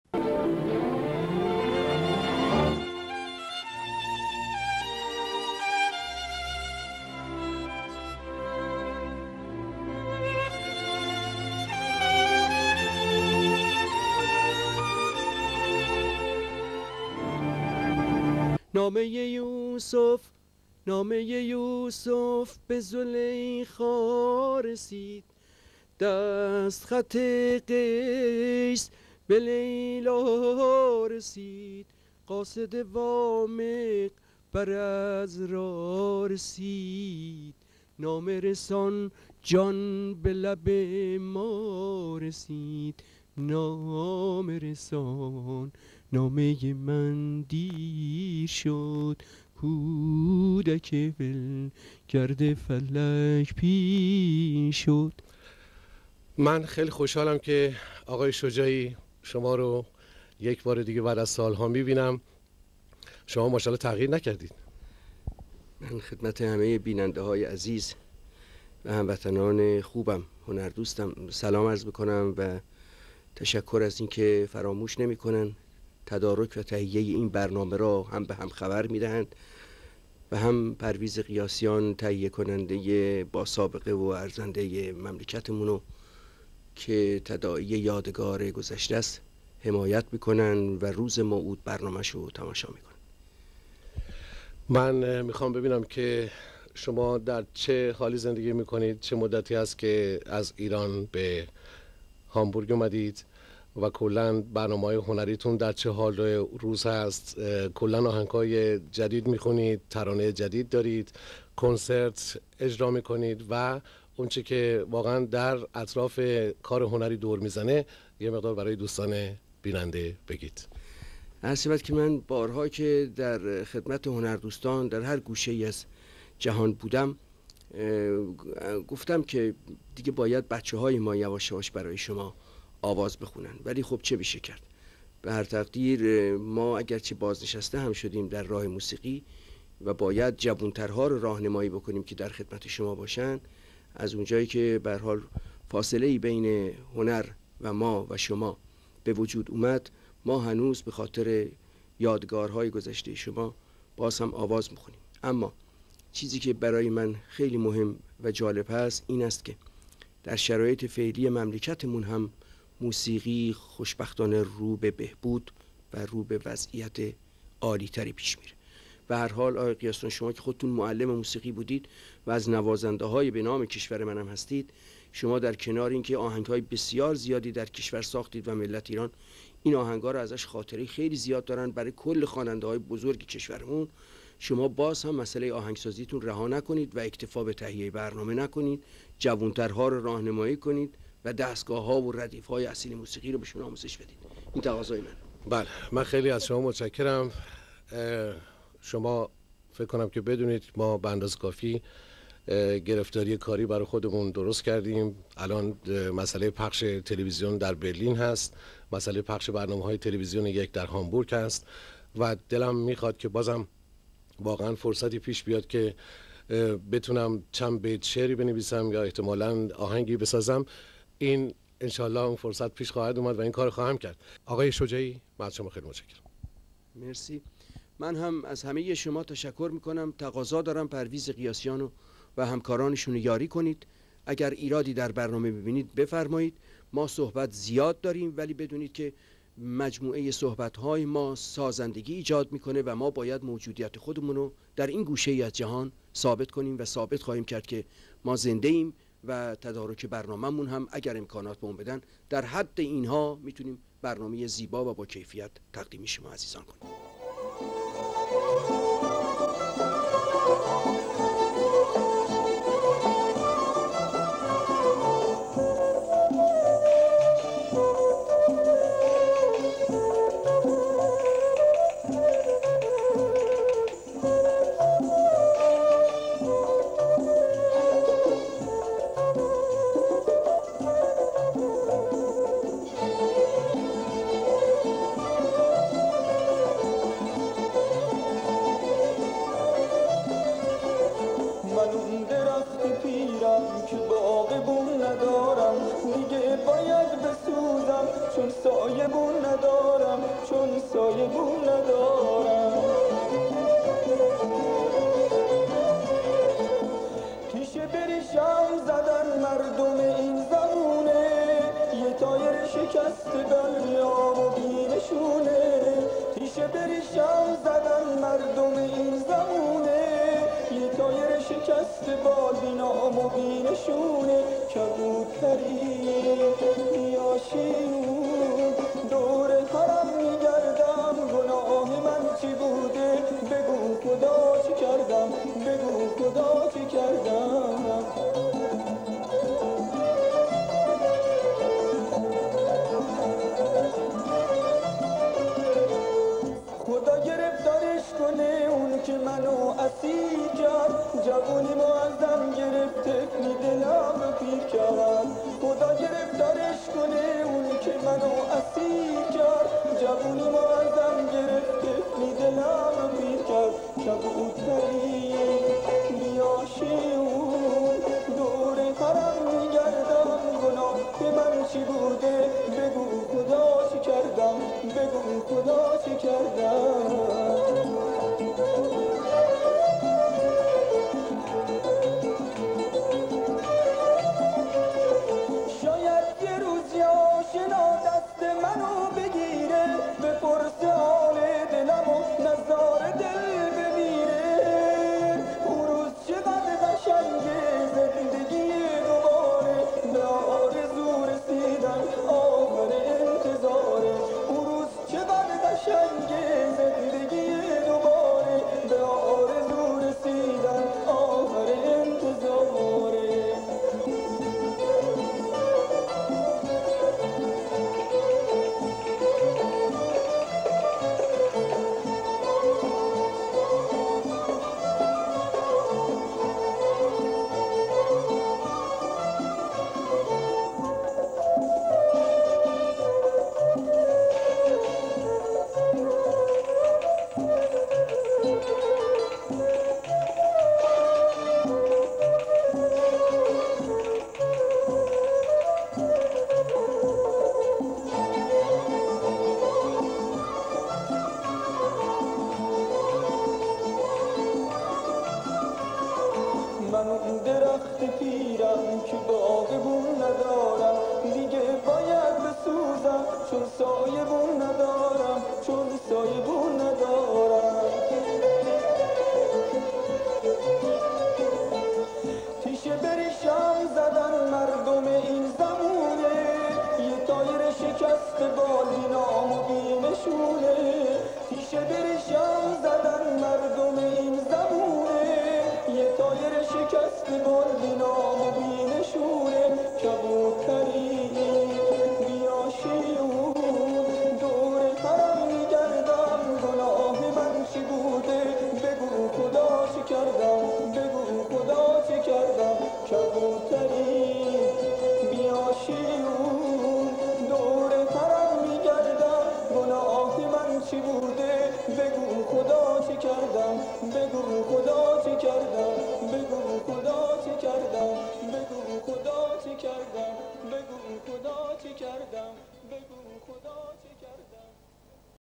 گفتگو با هنرمند عزیز حسن شجاعی پخش چهارم جولای 1991
بخشهای خاطره انگیز از 25 سال پخش برنامه های تلویزیون یک در آلمان